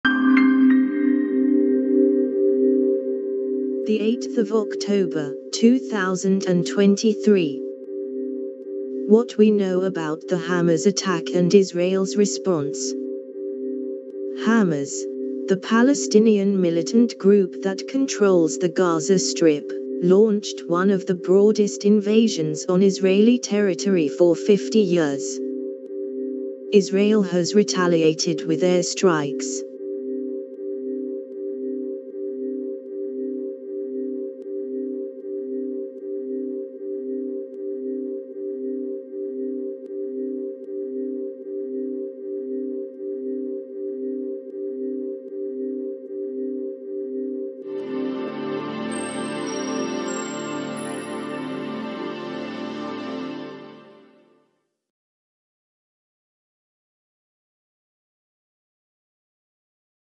[Latest]What We Know About the Hamas Attack and Israel’s Response
[Contents]
Hamas, the Palestinian militant group that controls the Gaza Strip, launched one of the broadest invasions on Israeli territory for 50 years. Israel has retaliated with airstrikes.